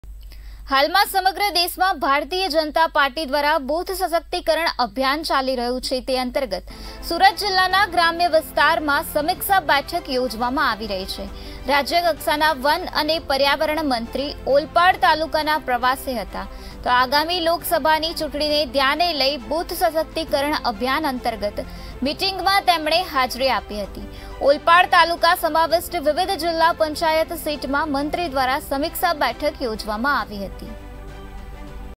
સમગ્ર દેશમાં BJP દ્વારા બુથ સશક્તિકરણ અભિયાન | Booth empowerment campaign by BJP across the country
સમગ્ર દેશમાં BJP દ્વારા બુથ સશક્તિકરણ અભિયાન
સુરત જિલ્લાના ગ્રામ્ય વિસ્તારમાં સમીક્ષા બેઠક યોજાઈ
પંચાયત સીટમાં મંત્રી દ્વારા સમીક્ષા બેઠક યોજાઈ